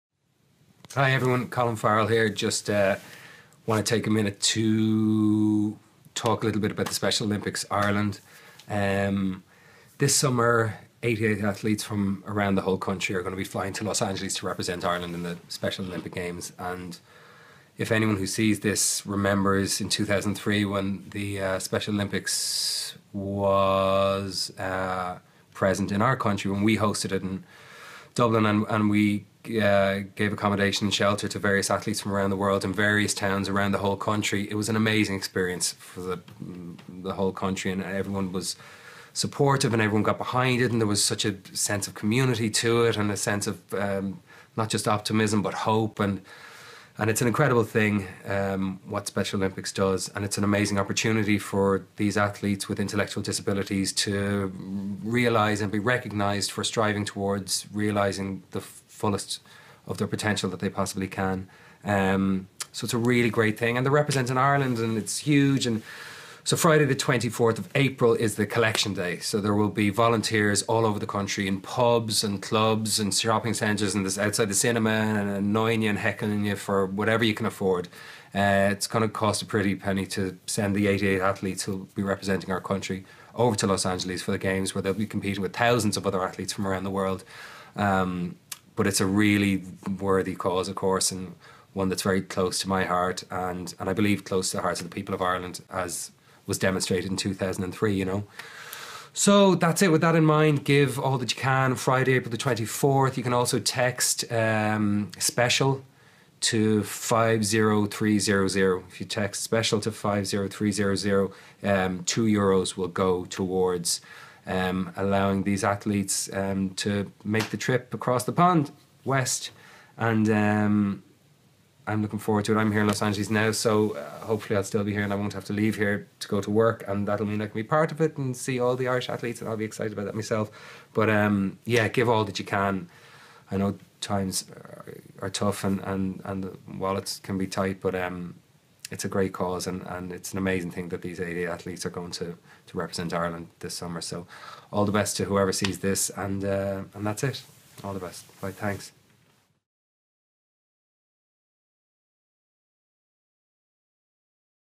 Colin Farrell supports Special Olympics Ireland
HOLLYWOOD actor Colin Farrell has called on Irish people to dig deep in support of Special Olympics Ireland annual Collection Day today, Friday 24th April. Every cent counts for the sports charity as it seeks to raise €500,000 in 24 hours. 3,500 volunteers will be shaking buckets in 200 towns and villages over the course of the day.
Support the Special Olympics Collection Day by texting the word ‘Special’ to 50300 to donate €2.
T&C’s apply. Some operators apply VAT which means a min of €1.63 will go to Special Olympics. for full information.